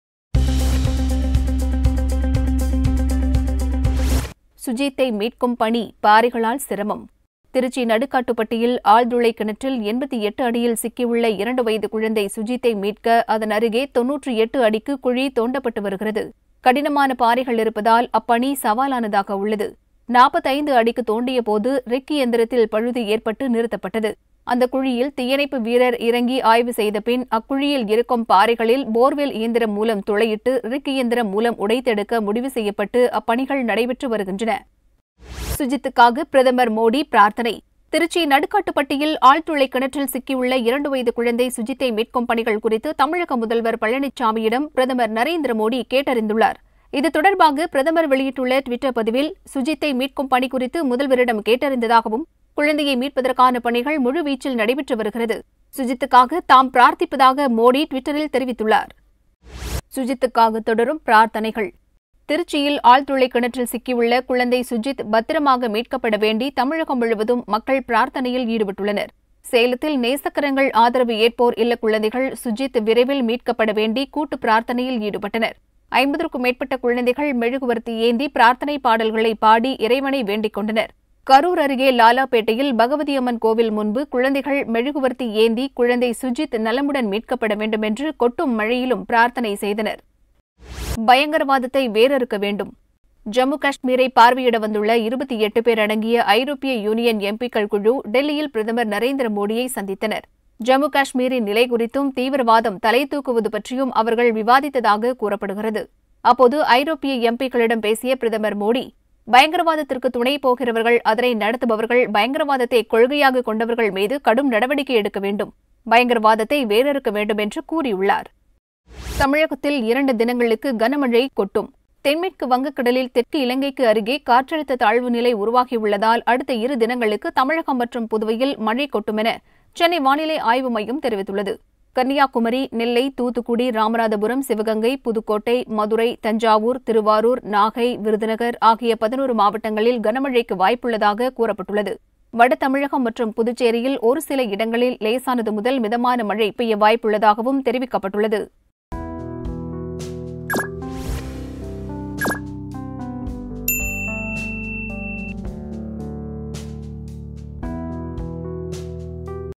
செய்திச்சுருக்கம் | Seithi Surukkam 28-10-2019 | மாலை 4 மணி | Dinamalar
1. சுஜித்தை மீட்கும் பணி; பாறைகளால் சிரமம்
2. சுஜித் மீட்பு பற்றி கேட்டறிந்த மோடி
3. சுஜித்துக்காக தொடரும் பிரார்த்தனைகள்
4.பயங்கரவாதத்தை வேரறுக்க வேண்டும்